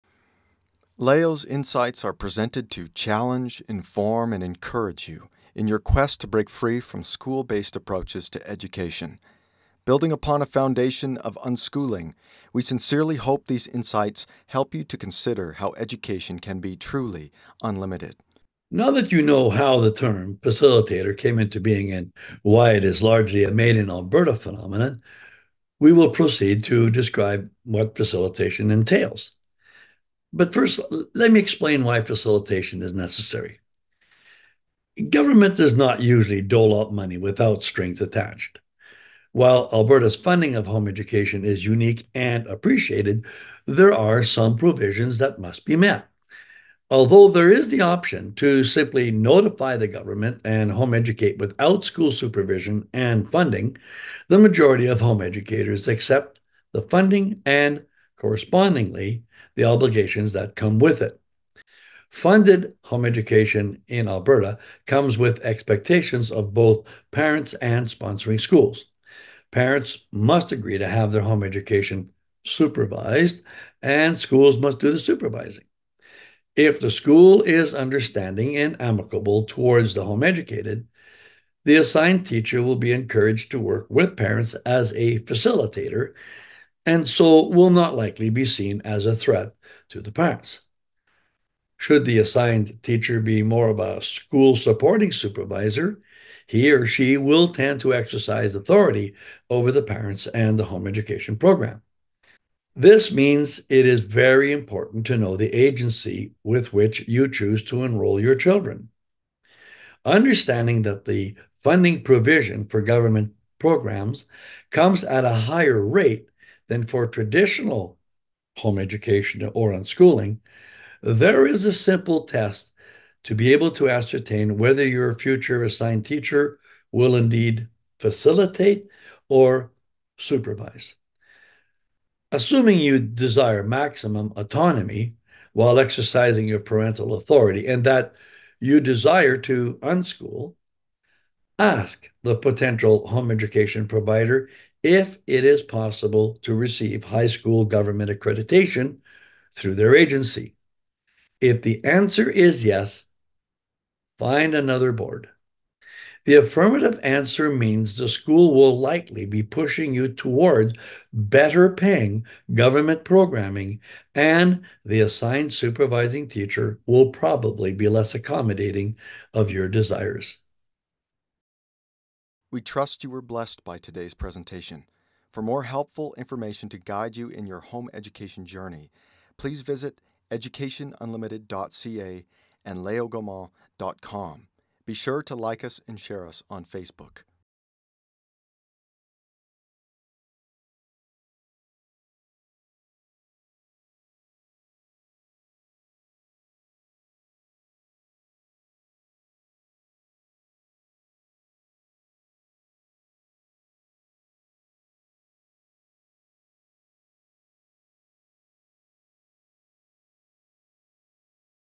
Facilitation - History Pt. 5: Back to the Basics (Part 34) by Léo Gaumont
In this video, Léo Gaumont explains that funded home education in Alberta comes with expectations of both parents and sponsoring schools. Parents must agree to have their home education “supervised” and schools must do the supervising.